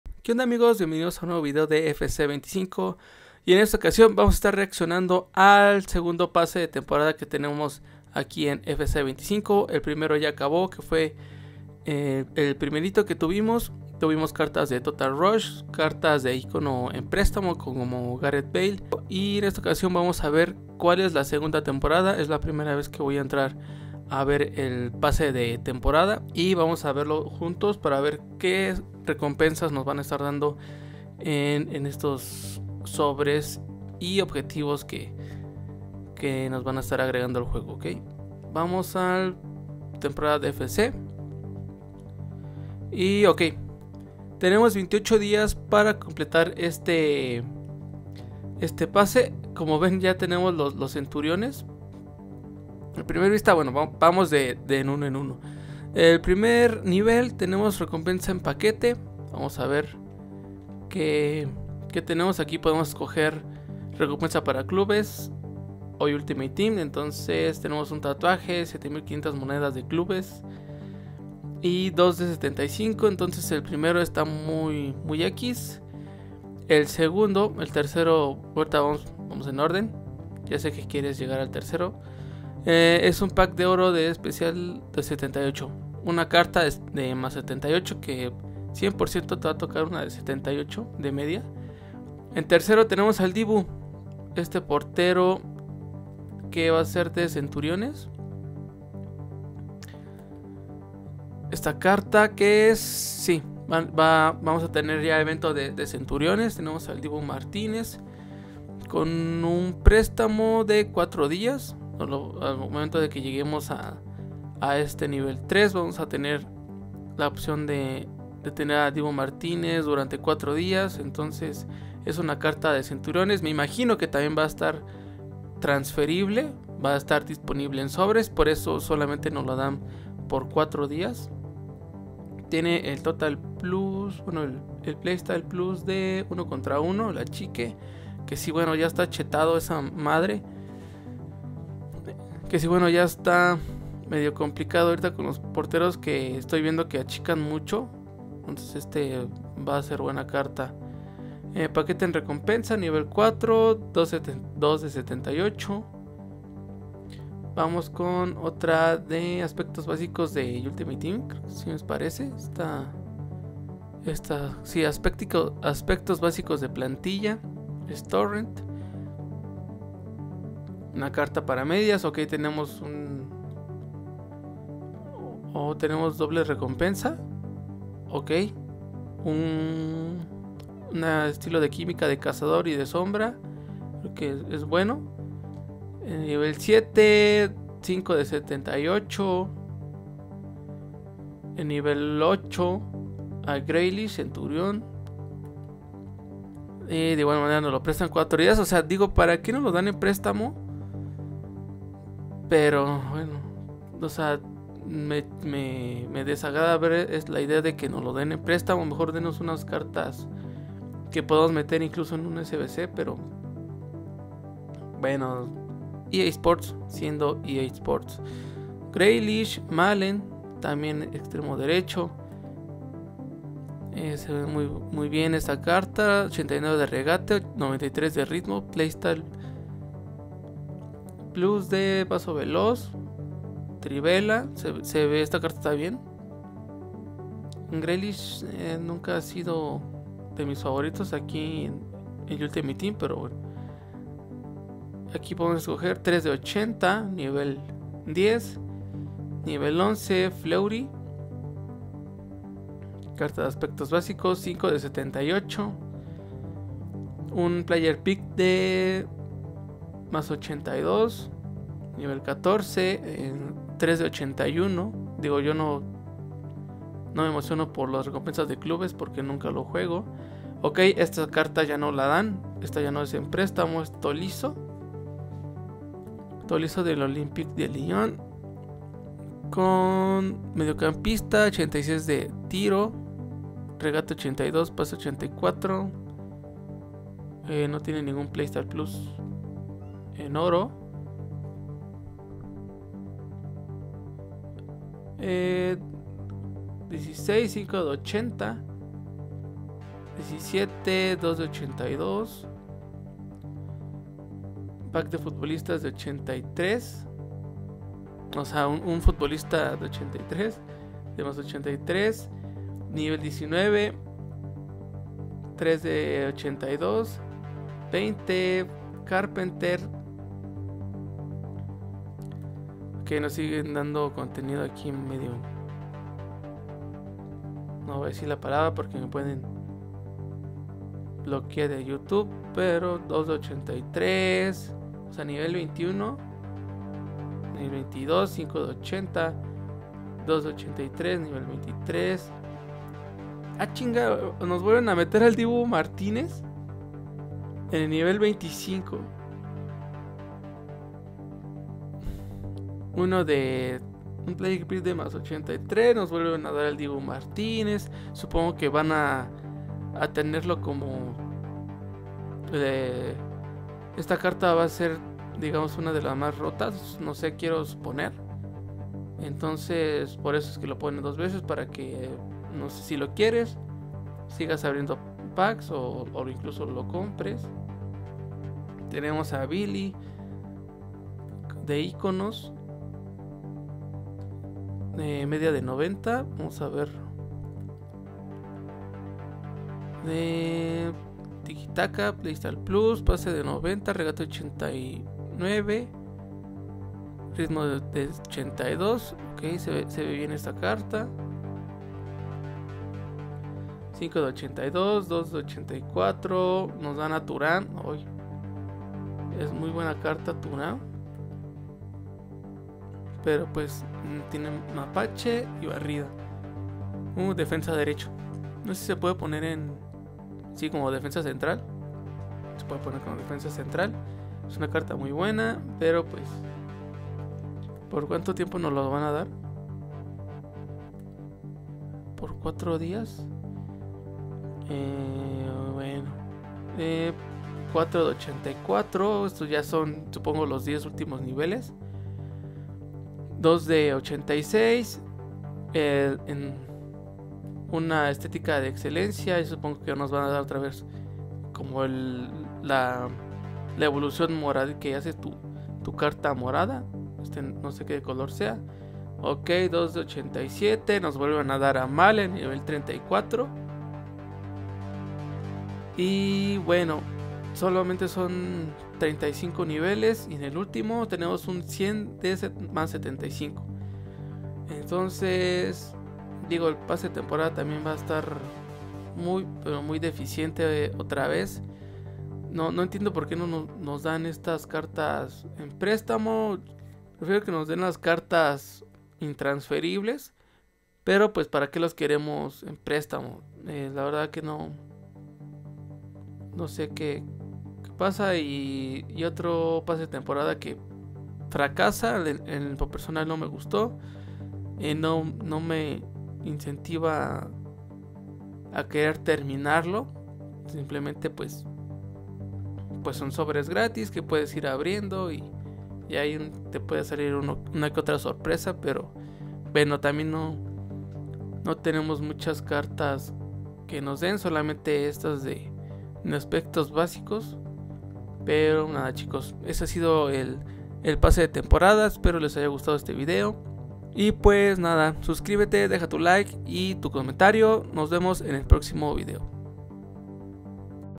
EA SPORTS VUELVE A DECEPCIONARNOS | FC25
Reaccionando al nuevo pase de Temporada de FC 25 de Noviembre.